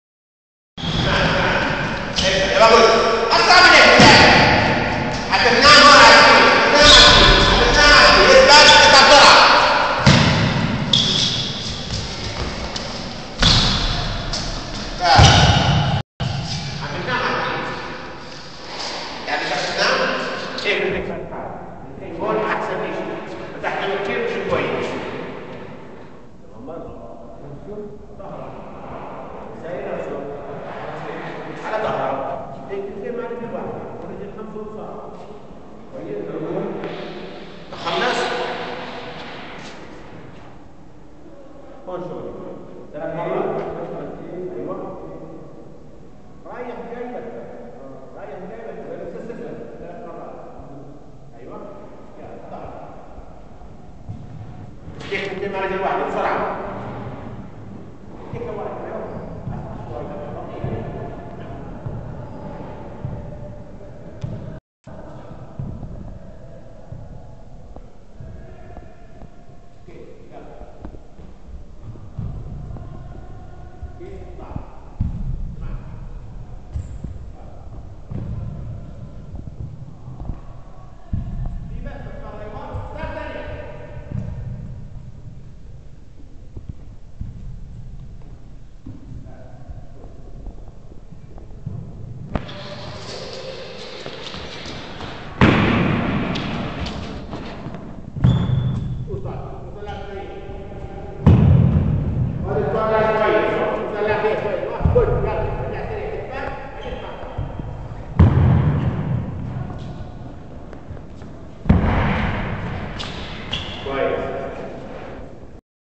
Ahmad Alsarraj (Jordan) Training Sessions with Dr. Ghazy (P6 of 12)
some training sessions of Ahmad Alsarraj, a junior squash pro from Jordan while training with one of Jordans best known celebrity trainers Dr غازي الجيلاني (currently involved with Wihdat Soccer Club)